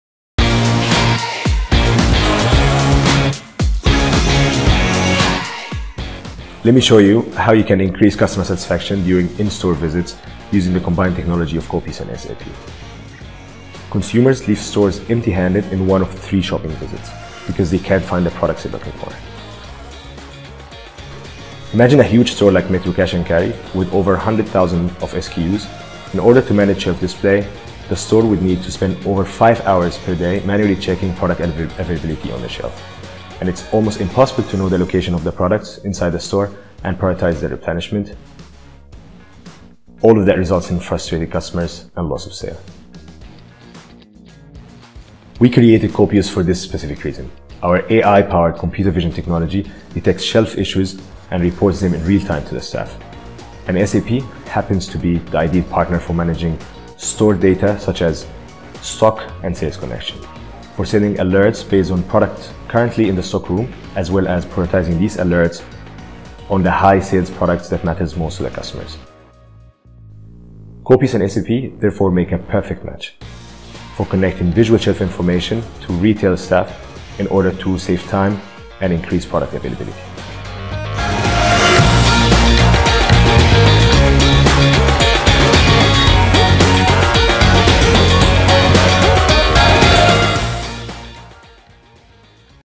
Digitize shelves using AI-powered computer vision from Qopius
Hear from Bedeir Rizk about how Qopius helps increase customer satisfaction by digitizing shelves.

Qopius is a graduate of SAP.iO Foundry Paris: Retail and Consumer Products focused B2B Tech (2019) Cohort.